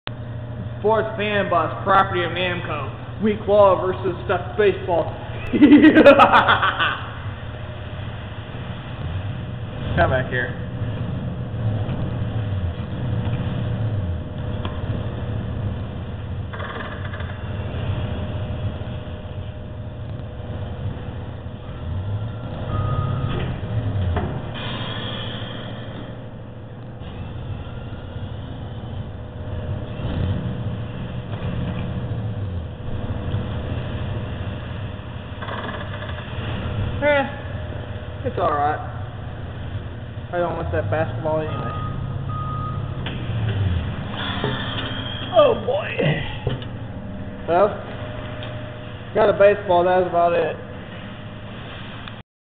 Sports Fan Bus CLaw Machine@ Walmart weak claw vs stuffed baseball
another vid from 2010 this was filmed at my local walmart it was before my chnnel was even created so i still hope you enjoy this